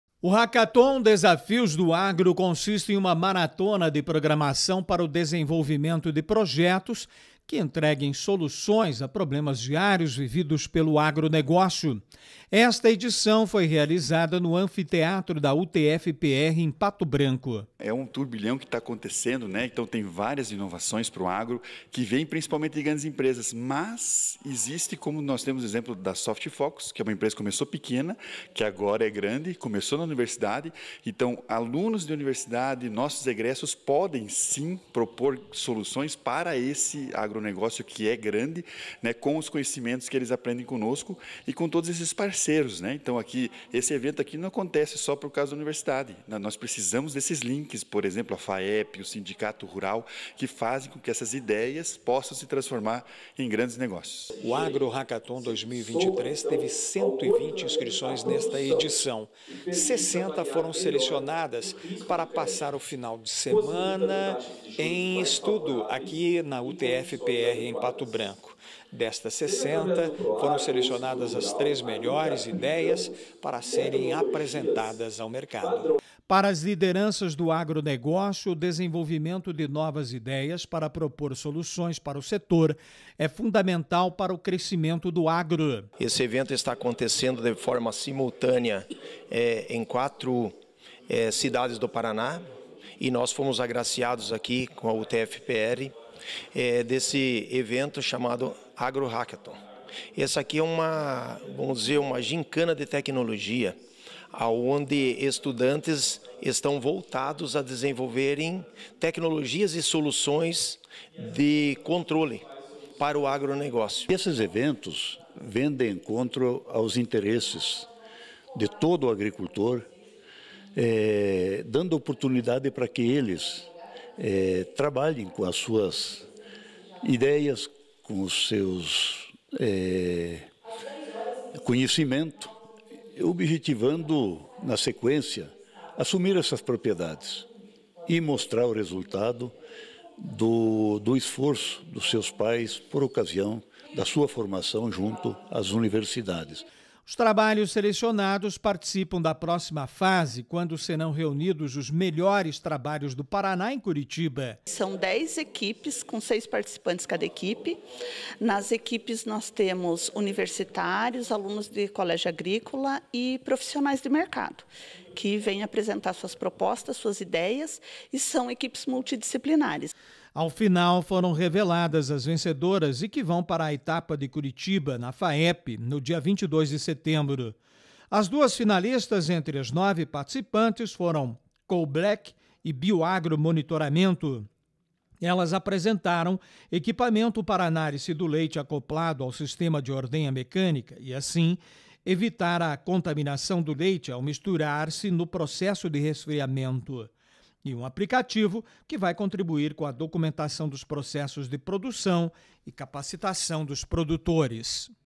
Indicados duas equipes para a final do Hackathon do Agro
Agro: Depois de dois dias de imersão tecnológica, Pato Branco indicou duas equipes para a final do Hackathon do Agro que será realizada dia 22 de setembro em Curitiba.